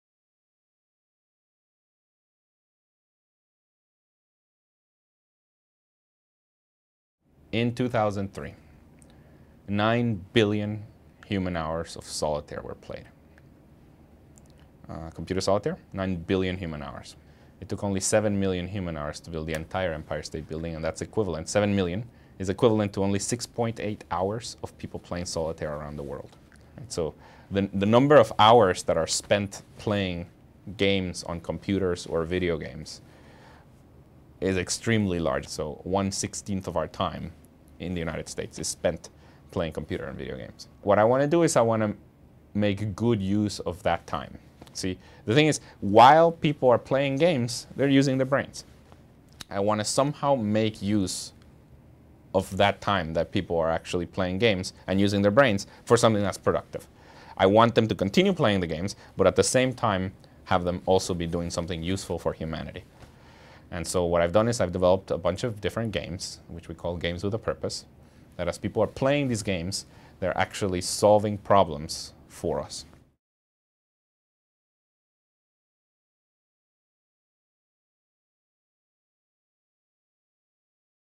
Games with a Purpose
Professor of computer science Luis von Ahn is getting people to solve problems that computers can't...while they play games.

From the "Inspire Innovation" areas of excellence videos - for more about Carnegie Mellon's comprehensive campaign,